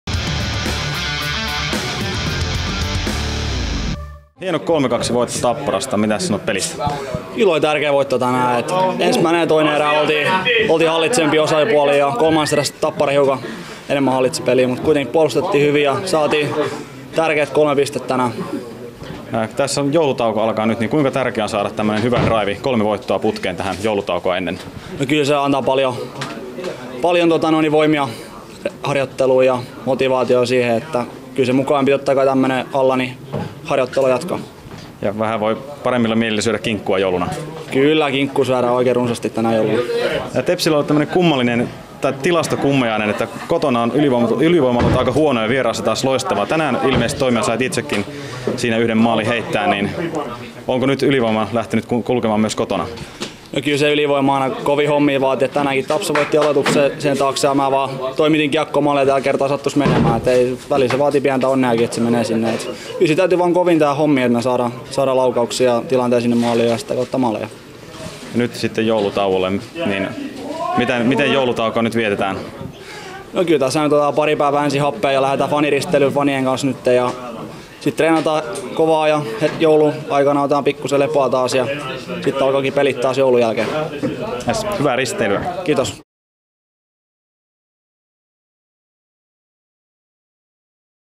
10.12.2011 Haastattelu: Markus Palmroth
Tärkeät kolme pistettä taskussa on hyvä lähteä faniristeilylle ja sen myötä joulutauolle. Markus Palmrothin pukukoppihaastattelu Tappara-voiton jälkeen.

Toimittaja: Henri Saloranta
Kuvaus: Aleksi Myllylä